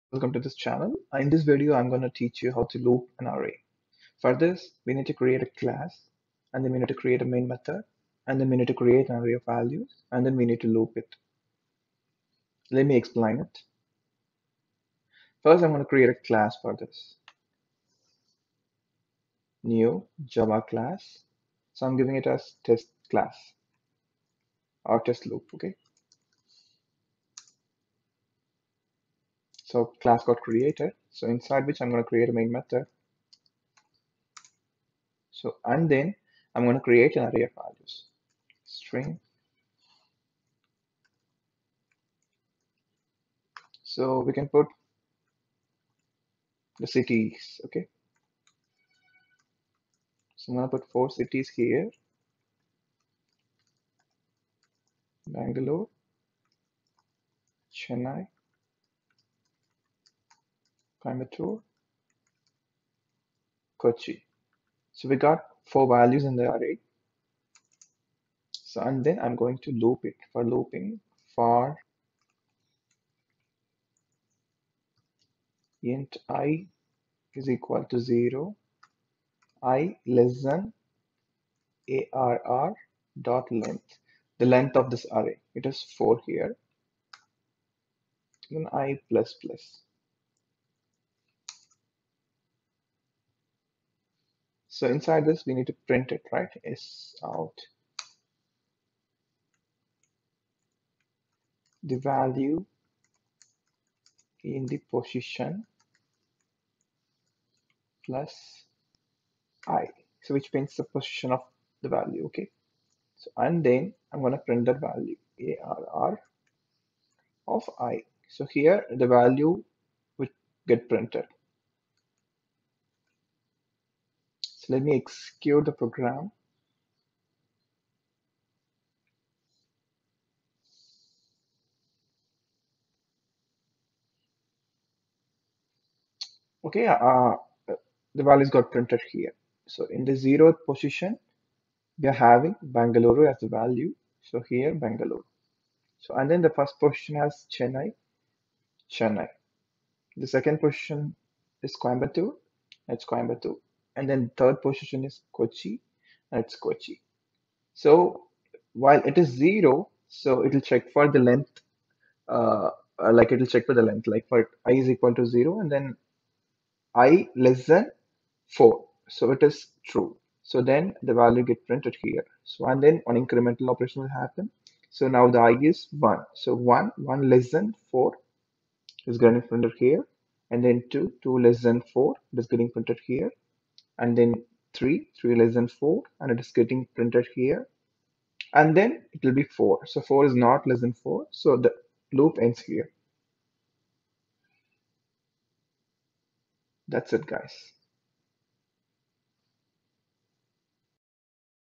Java Interview Question| Java Tutorial for Beginners | How to loop an array| HomeWorkCodes
This video is to understand how to loop an array of values in java.